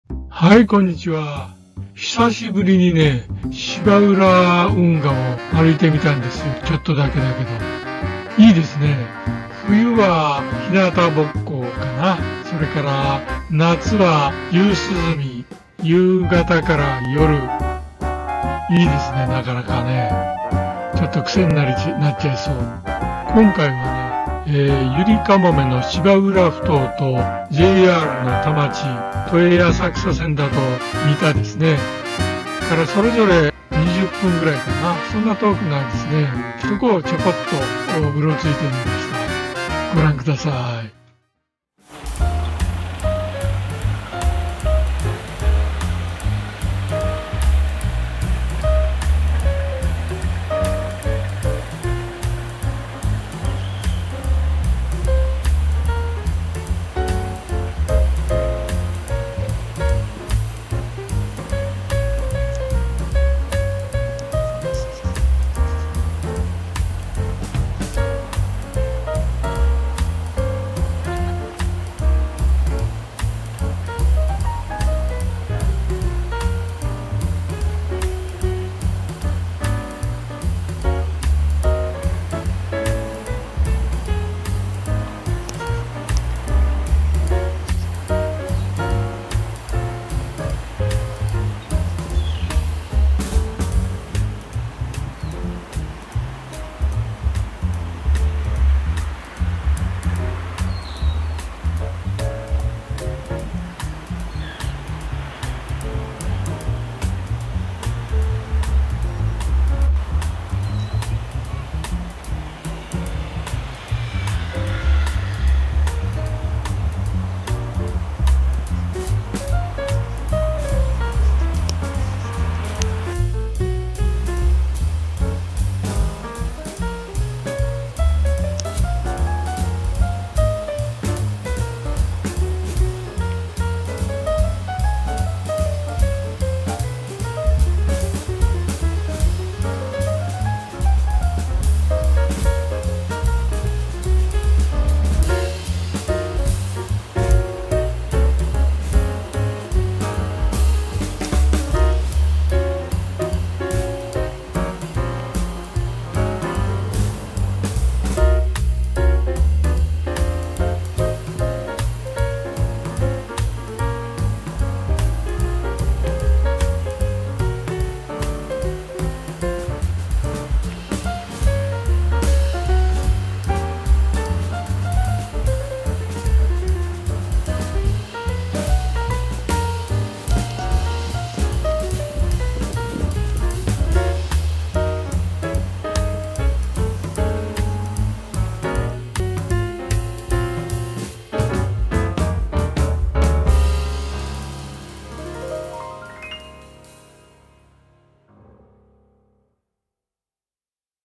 【東京都心】港区：芝浦運河散歩
久し振りに芝浦運河へ行ってみました。
なかなか良いですネ！ノンビリユッタリ散歩にちょうどイイ
ベンチもけっこう多くあって休んでもいられるしネ！
冬はひなたぼっこ、　夏は夕涼み、春と秋は　いつでもネ